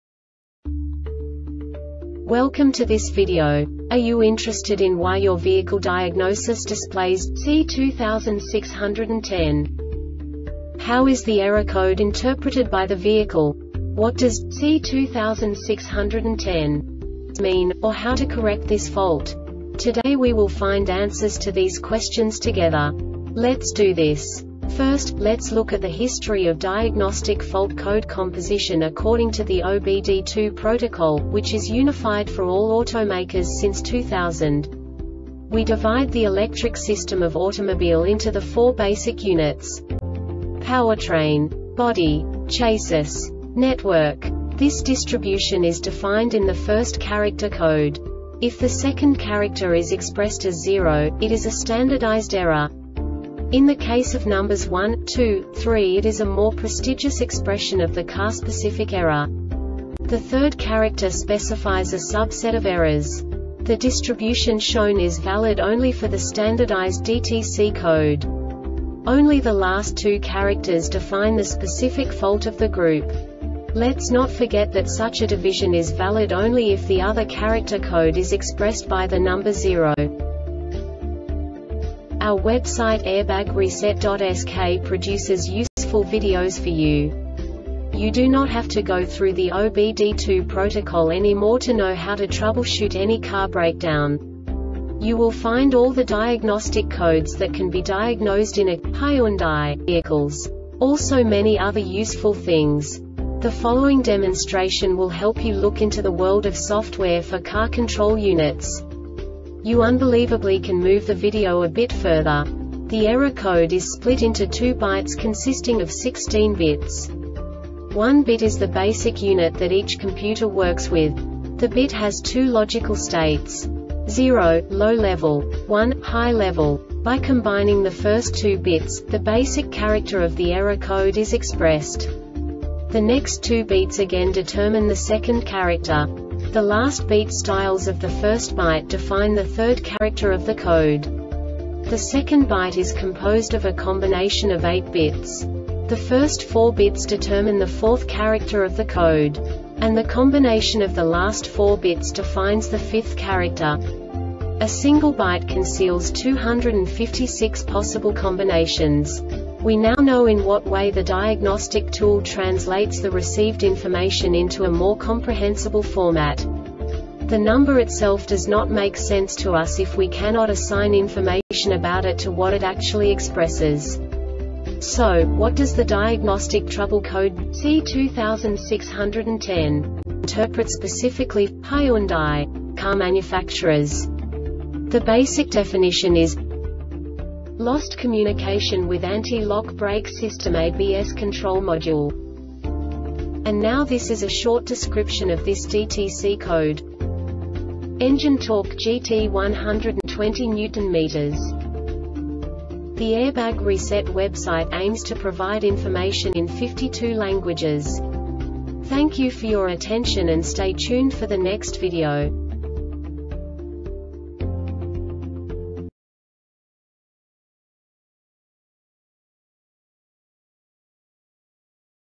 DTC Hyundai C2610 Short Explanation
Contents:
0:21 Basic DTC analysis according to OBD2 protocol standard.
1:48 Insight into programming
2:58 A diagnostically understandable description of the Diagnostic Trouble Code
3:05 Basic error definition

Make:
Hyundai
Code:
C2610
Definition:
Description:
engine torque &gt;120Nm